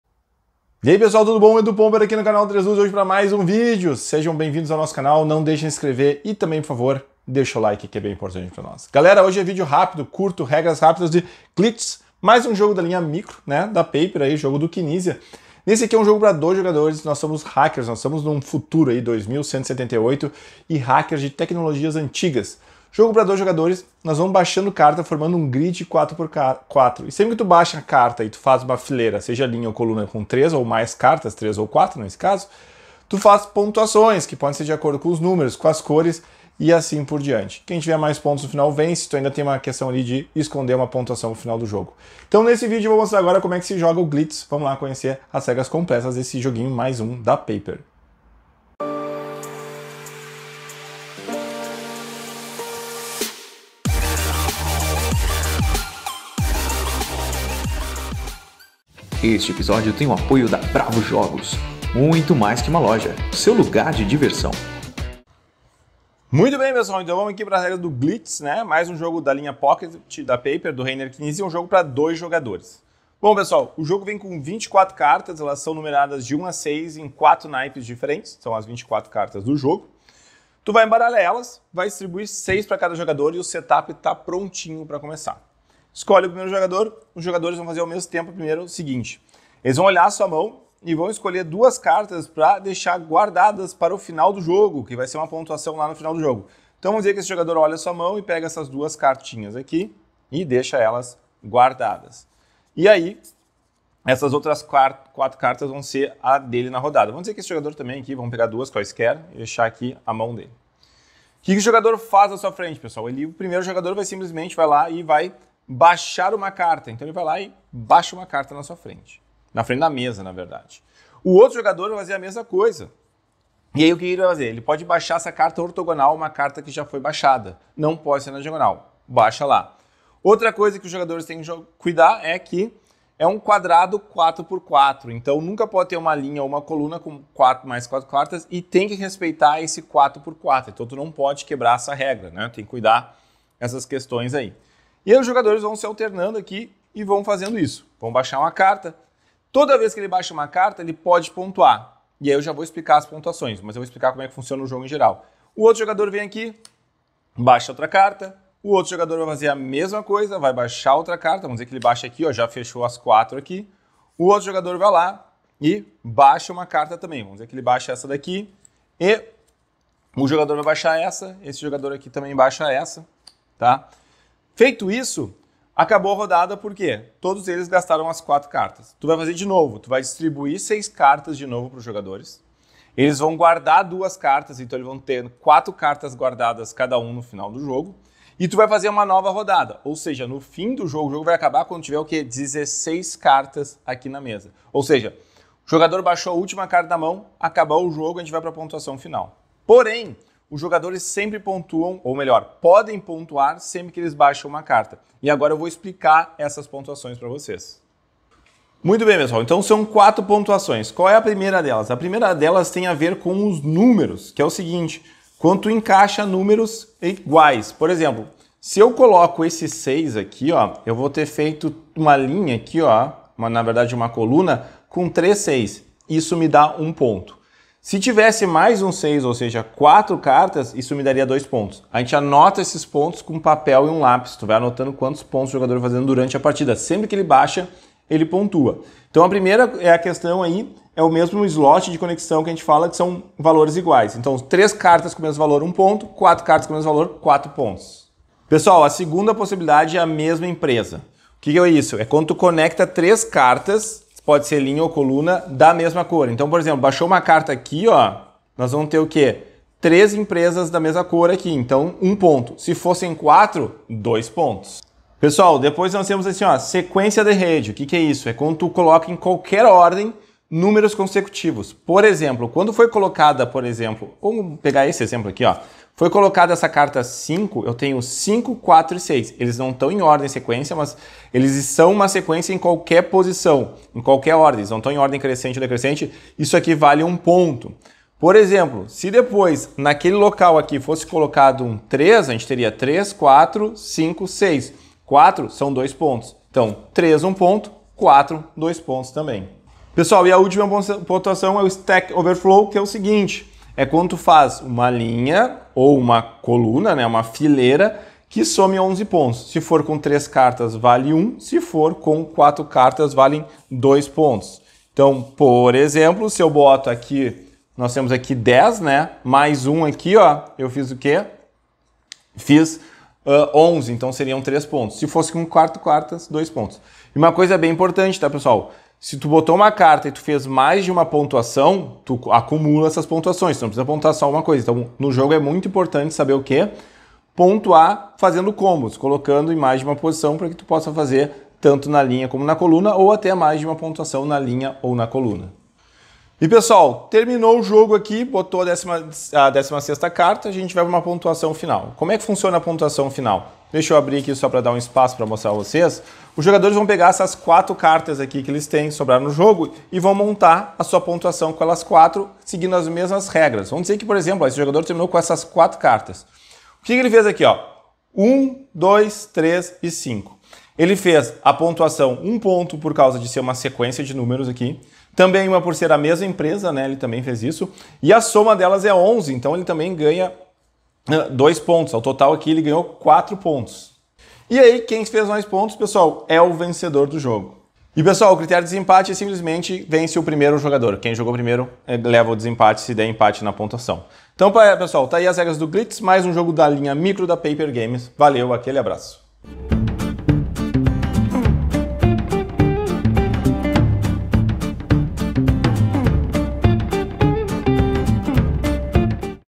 Glitches - Regras - Aprenda em Minutos
Glitches é o mais novo lançamento da PaperGames, mais um jogo da linha micro. Nesse jogo, de Reiner Knizia, os jogadores são hackers de tecnologias antigas no ano de 2178. Um jogo para dois jogadores, na sua vez os jogadores baixam uma carta no centro da mesa, onde vão formando um quadrado 4 x 4 de cartas. Sempre que alinham 3 ou 4 cartas eles pondem pontuar, existem 4 formas diferentes de pontuação.
Que saber as regras completas? Então dá o play no vídeo do Edu Pomper!